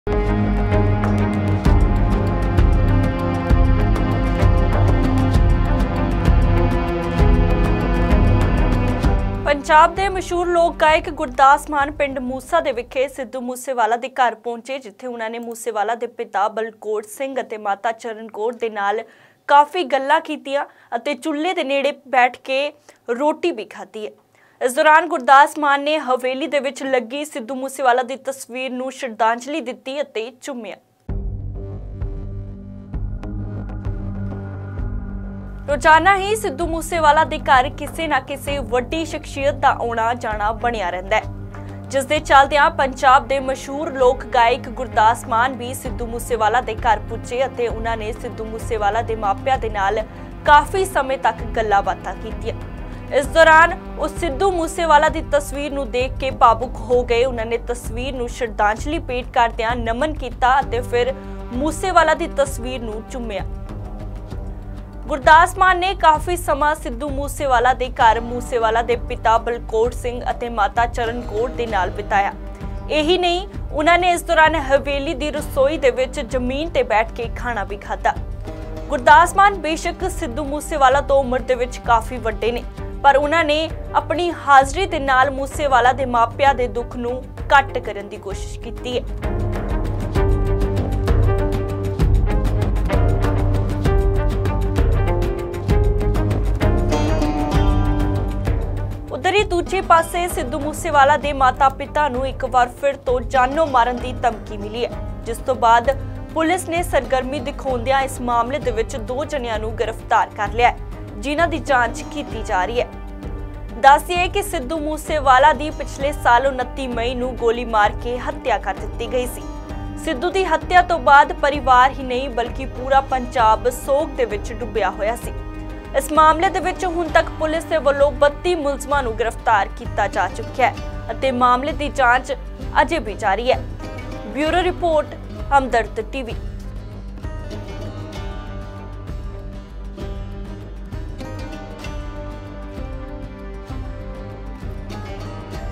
Sidhu Moosewala ਦੇ ਘਰ ਪੁੱਜੇ Gurdas Mann, ਮੂਸੇਵਾਲੇ ਦੀ ਤਸਵੀਰ ਨੂੰ ਗਲ ਲਾ ਕੇ ਚੁੰਮਿਆ, ਜ਼ਮੀਨ ’ਤੇ ਬਹਿ ਕੇ...
ਸਿੱਧੂ ਮੂਸੇਵਾਲੇ ਦੇ ਘਰ ਪੁੱਜੇ ਗੁਰਦਾਸ ਮਾਨ
ਮੂਸੇਵਾਲੇ ਦੀ ਤਸਵੀਰ ਨੂੰ ਗਲ ਲਾ ਕੇ ਚੁੰਮਿਆ
ਮਾਪਿਆਂ ਨਾਲ ਦੁੱਖ ਕੀਤਾ ਸਾਂਝਾ, ਜ਼ਮੀਨ ’ਤੇ ਬਹਿ ਕੇ ਖਾਧੀ ਰੋਟੀ